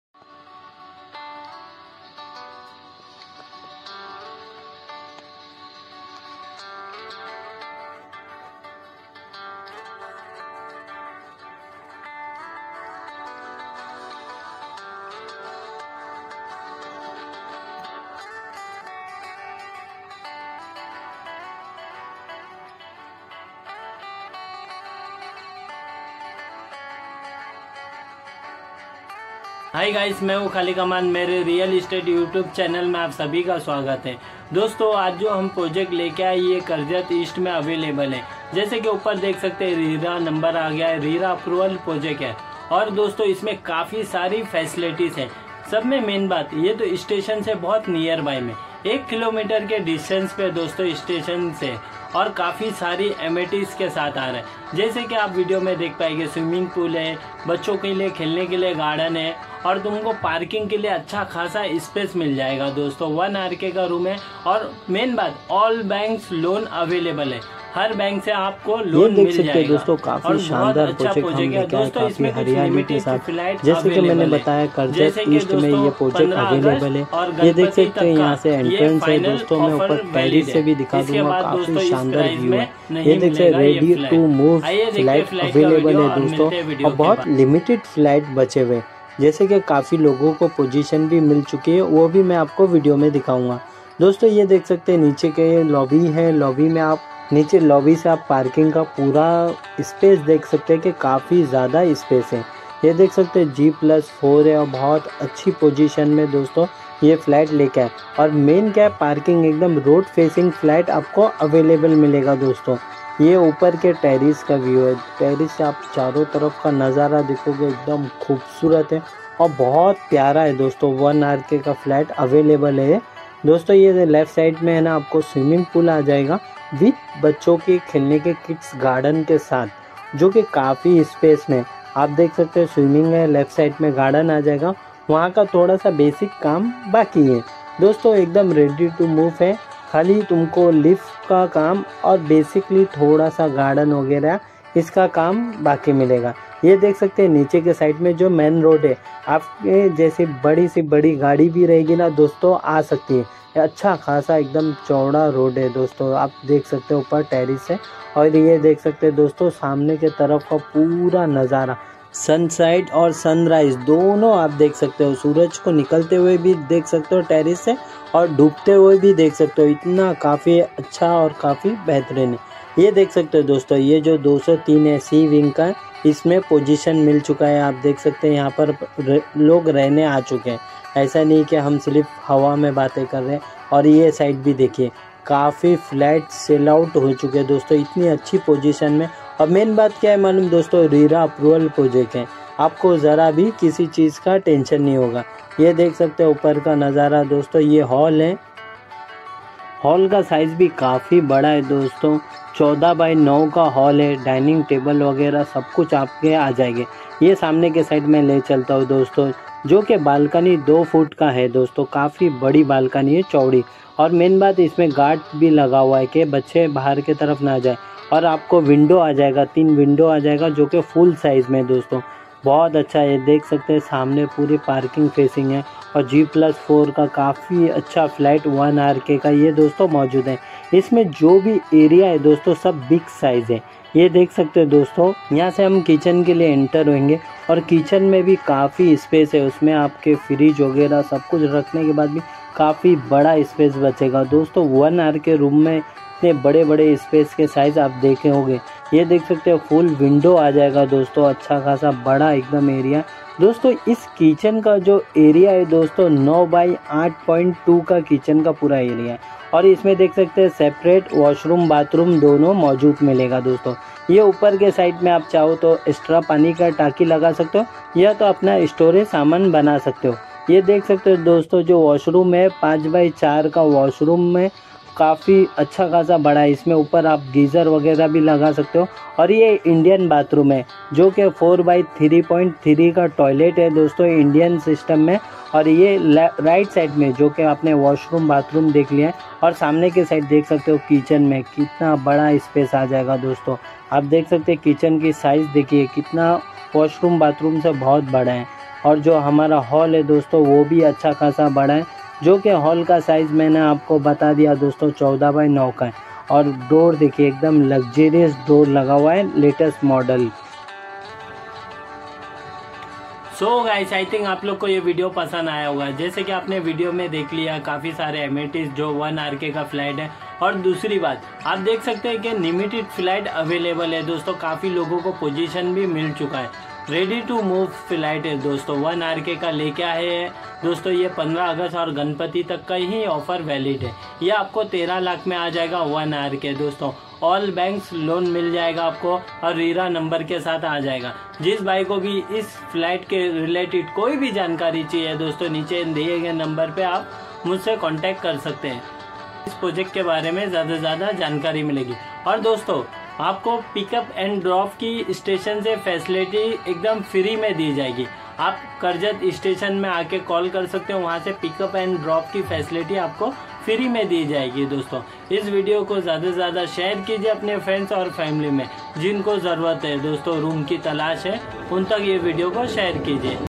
📢13 लाख 1 रियल एस्टेट फ़्लैट दूरी 1 किमी कर्ज़ 1RK flat distance 1km karjat
karjat

                    MAHARERA REGD NO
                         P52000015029

 STILT+4 floor with lift 3 Acer

hi guys ya jo project  hai bhot shandar project Hai jismain 1RK 1BHK flat available hai lowest price jis bhai ko bhi Lena Ho mujhse

 ( 1 )   CCTV.
 ( 2 )   security guard
 ( 3 )   kids play garden
 ( 4 )  telephone intercom
 ( 7 )  high  speed lift 🛗
 ( 8 )  Mandir
 ( 9 )  fitness gym

*NEAR BY PROJECT #*

 Karjat Station  ( 5 mins Drive )
 10 min walking distance Karjat Station

Near Upcoming Reliance University which is 1200 Acres

4 College Near Project Name -  Kokan Gyanpeeth
Engineer College,  IBSAR Management College Dr.Abdul Kalam College
DON Bosco Universal school
Near By Hospital ,Multiplex , Market , Schools, College

Navi Mumbai International Airport ( 30km )
Mumbai-Pune Express  Highway ( 20mins Drive )
D-Mart  (10 mins )
MATHERAN HILL STN (10 Km)
Celebrity Farm House  (5 km )
N.D.Studio Film City  ( 7km )
Up Coming  International Greg Norman  60 Hole Gold Course ( 7km ).